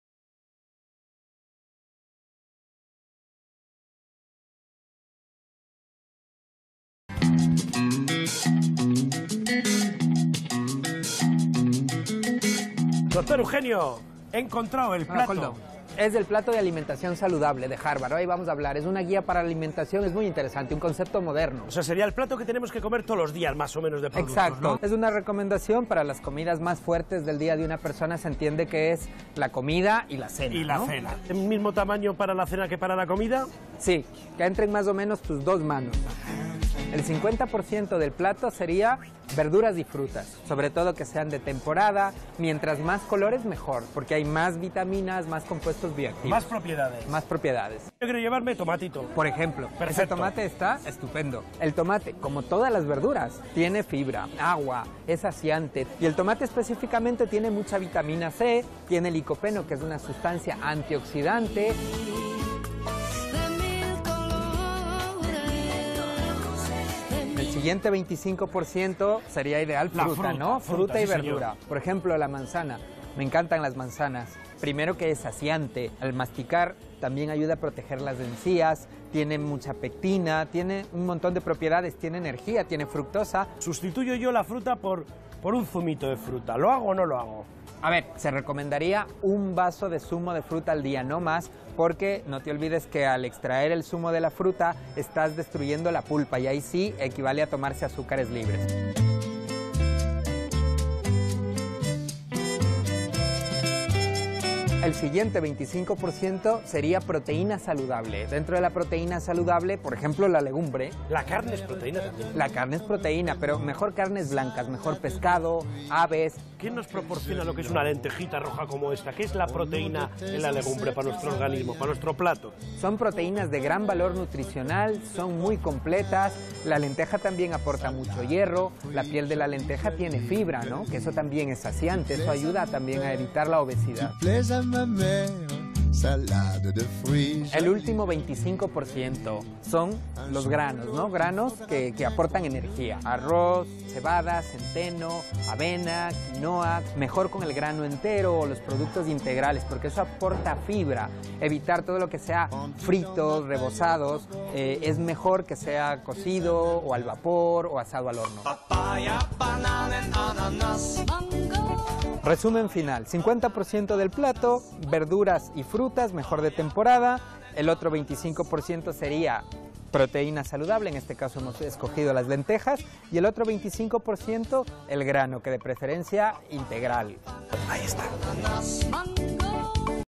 Alimentos que debe llevar un plato saludable
Llevar una alimentación variada y saludable puede ser bastante sencillo si seguimos los consejos del Dr. Eugenio Freire, especialista del Centro Médico-Quirúrgico de Enfermedades Digestivas.
El 50% del plato debe estar compuesto de verduras y frutas, un 25% debe estar constituido por proteínas de buena calidad, como las que están presentes en legumbres, carnes blancas y pescado, y el último 25%, debe representar los hidratos de carbono, por ejemplo, el arroz, y preferiblemente en su versión integral.